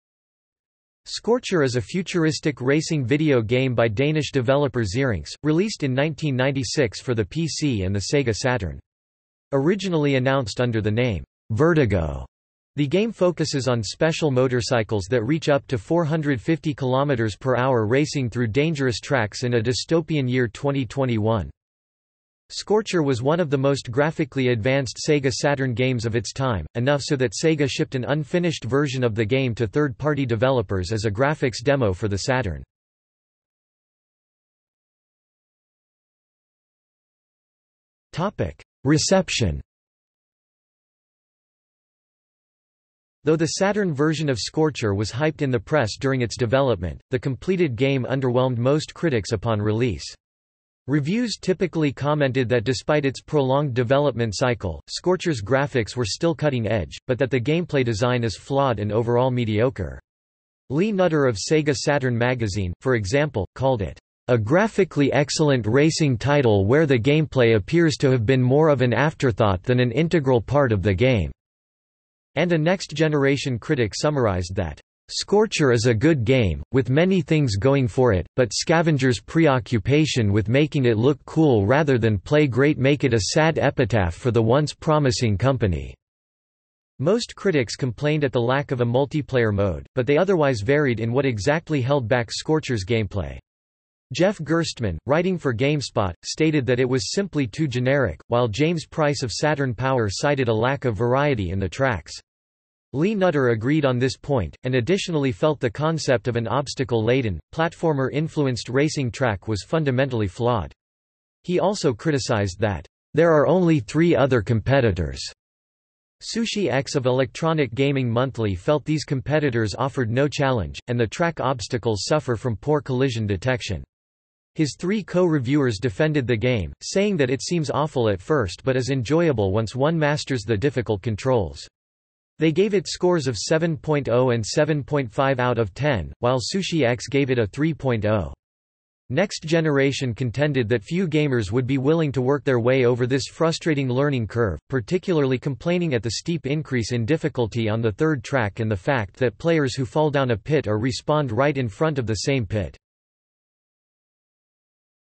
Scorcher (video game) | Wikipedia audio article
00:00:34 Reception

- Socrates

SUMMARY
Scorcher is a futuristic racing video game by Danish developer Zyrinx, released in 1996 for the PC and the Sega Saturn. Originally announced under the name "Vertigo", the game focuses on special motorcycles that reach up to 450 km/h racing through dangerous tracks in a dystopian year 2021.
Scorcher was one of the most graphically advanced Sega Saturn games of its time, enough so that Sega shipped an unfinished version of the game to third party developers as a graphics demo for the Saturn.